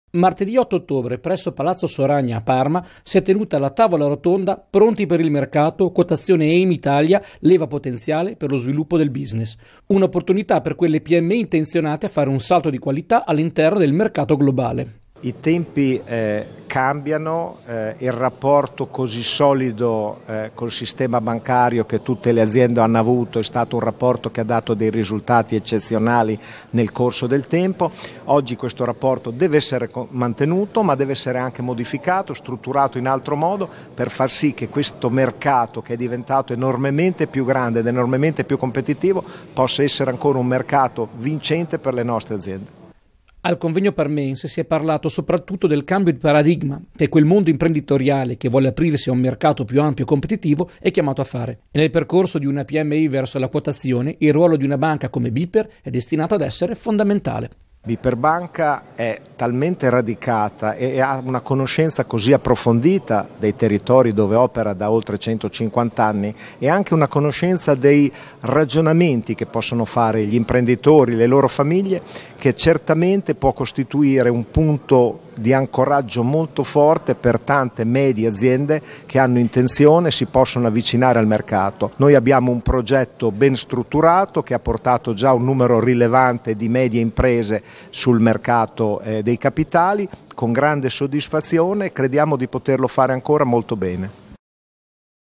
Bper tra imprenditoria e globalizzazione
A Parma martedì otto ottobre si è tenuta una tavola rotonda nella quale Bper e il mondo della imprenditoria si sono confrontati sulle prospettive offerte alle PMI dalla quotazione AIM Italia come leva per lo sviluppo del business nel contesto del mercato globale.